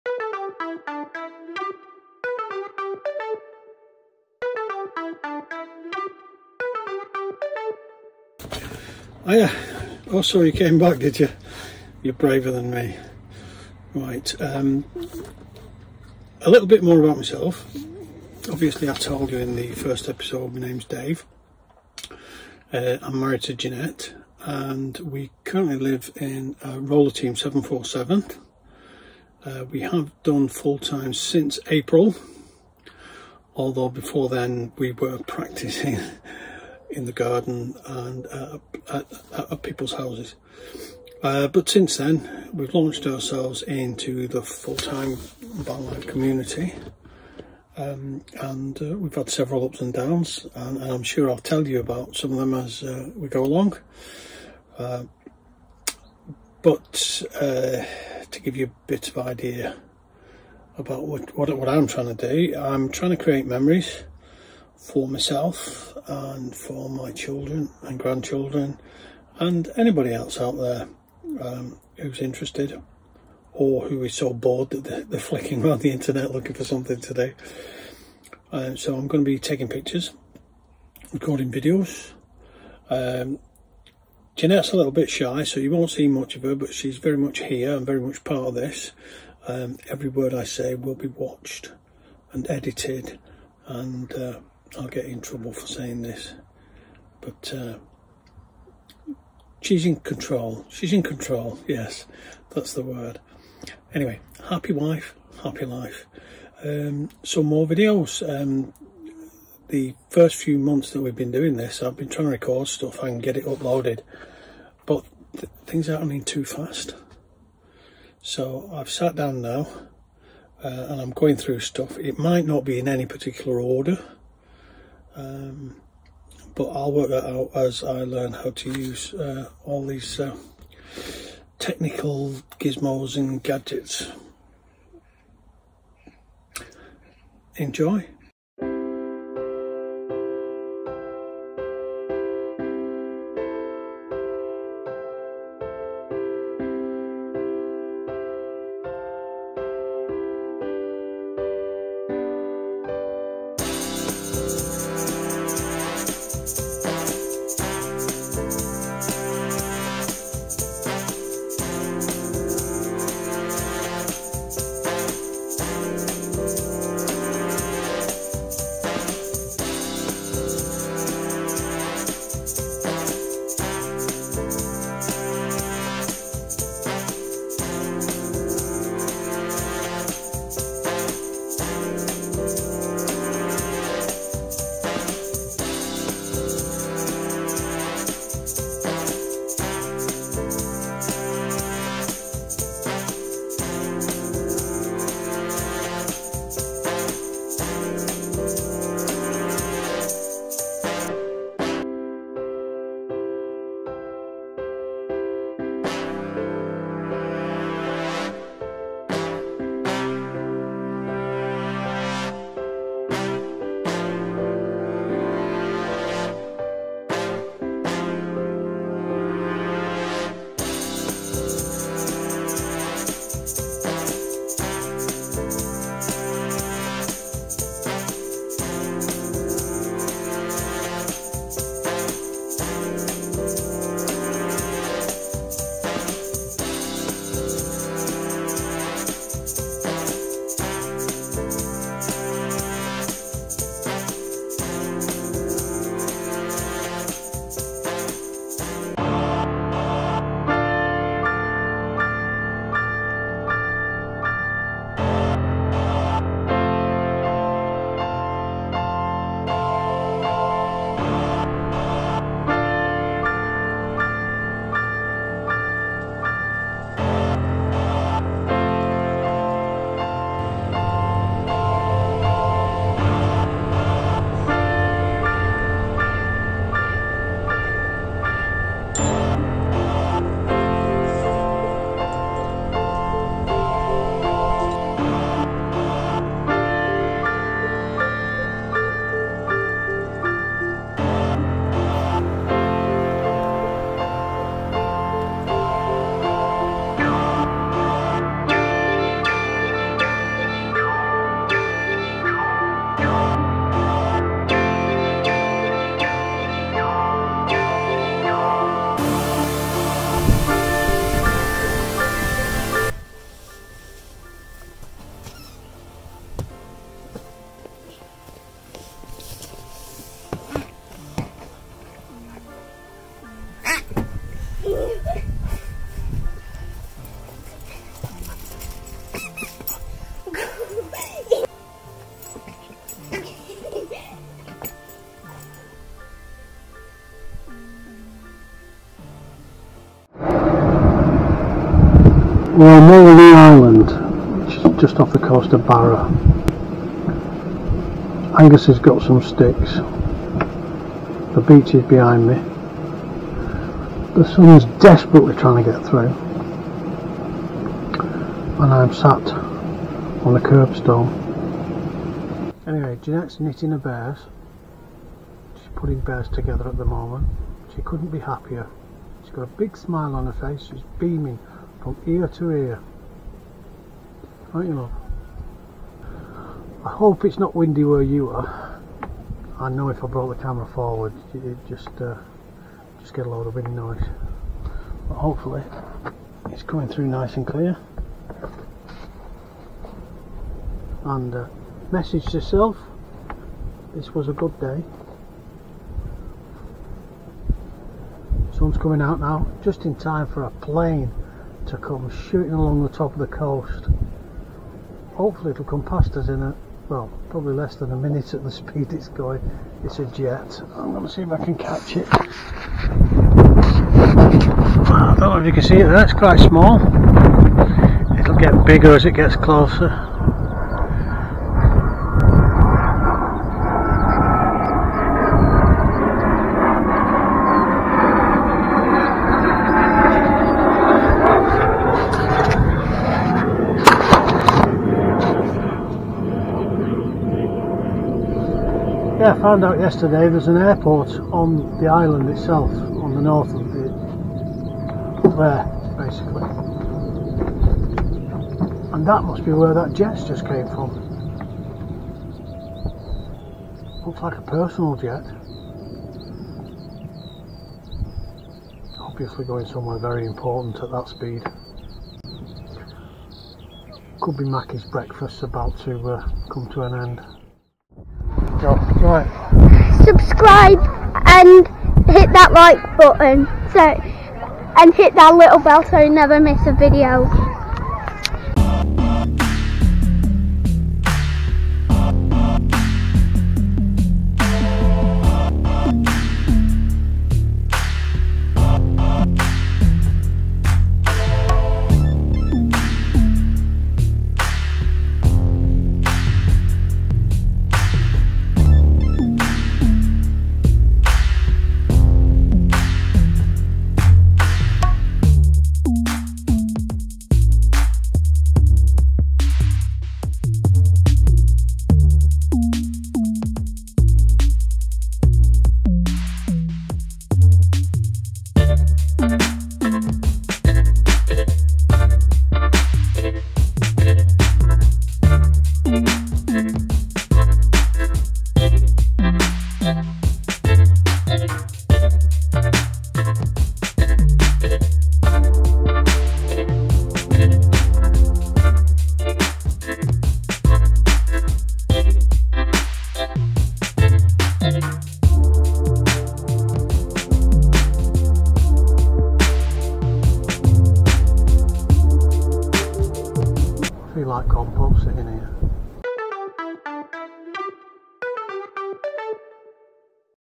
A little more about us
Footages the Kelpies, and Loch Ken in Scotland ,also some clips of Angus an Charlie ar Holbeach Hurn , and to close some pictures of Stonehenge  to some garage band tunes.Let us know what you think ta .